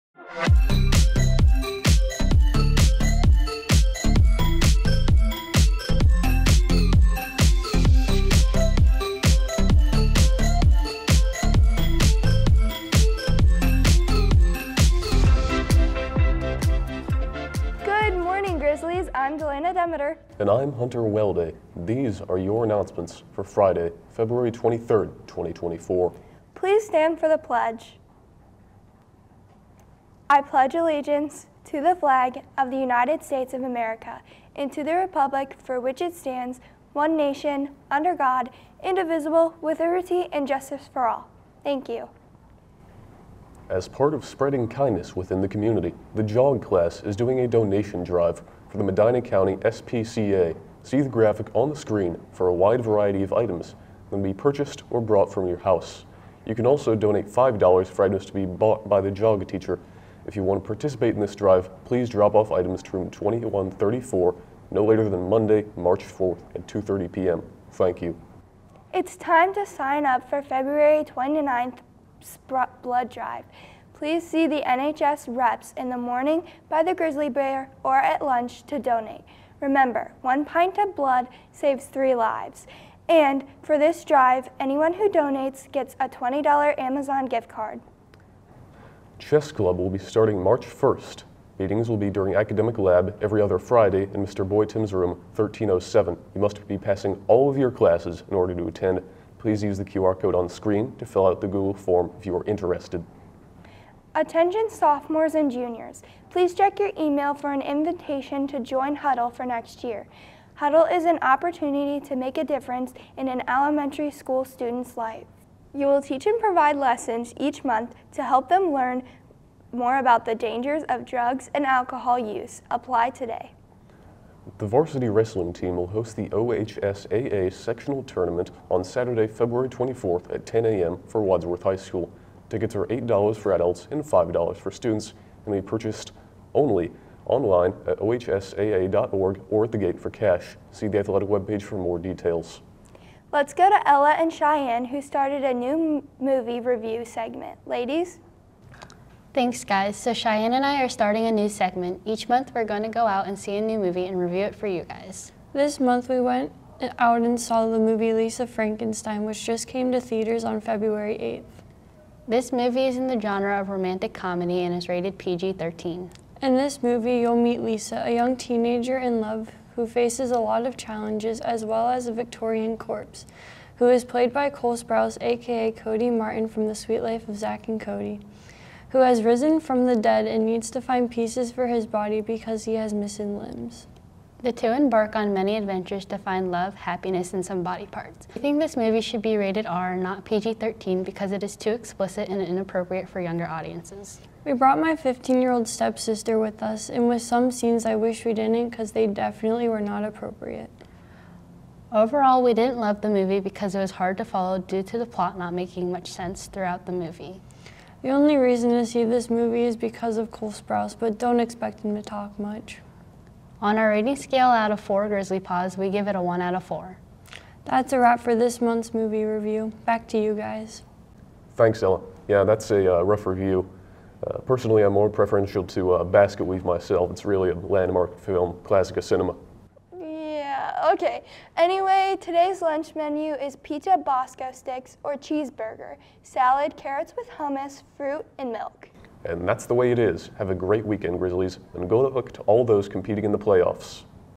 Good Morning Grizzlies - 2/23/24
Catch all your WHS news and a movie review of Lisa Frankenstein.